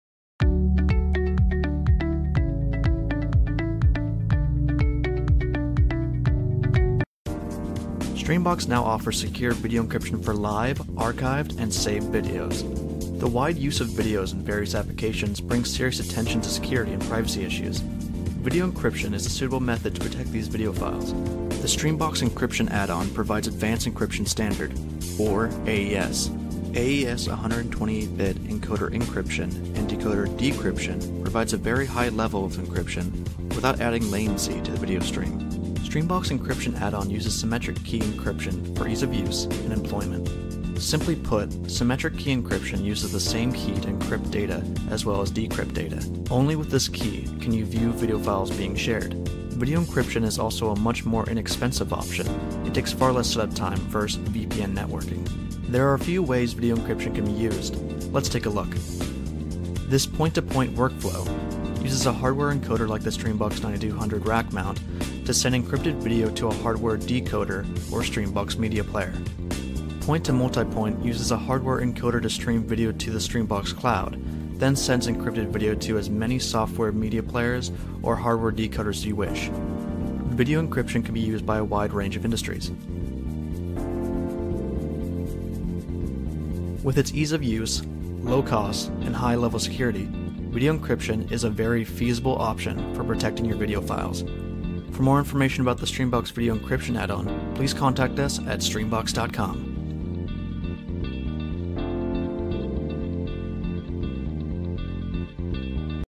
Video Encryption for Live streaming and files
Streambox now offers secure video encryption for live, archived, and saved videos. The wide use of videos in various applications brings serious attention to security and privacy issues. Video encryption is a suitable method to protect this data.